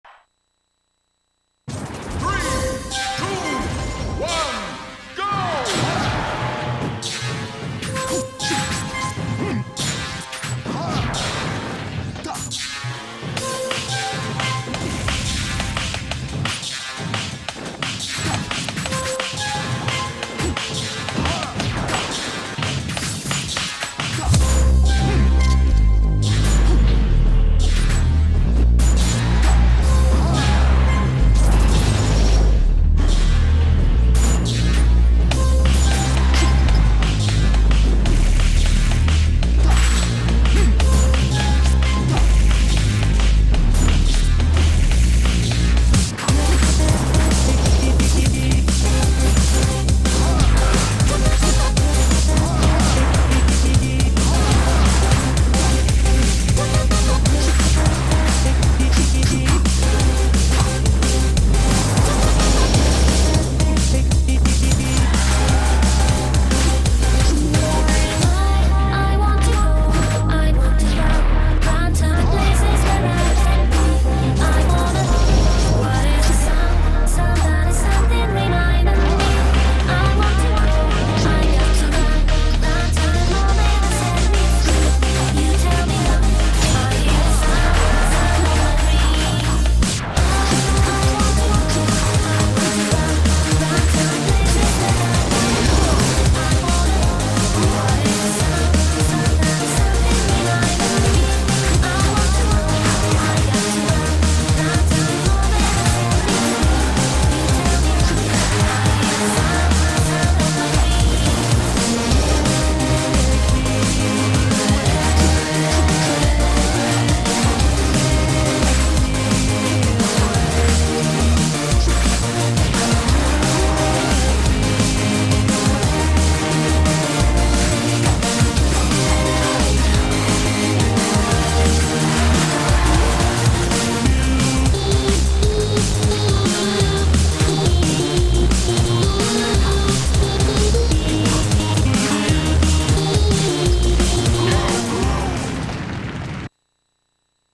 BotIE2 WF - Holy Nightmare (MK) vs Ally (MK) 2
Singles Bracket at Battle of the Ice Empire 2 - Tin Man on Ice, April 16th, 2011. Hosted by KingKong.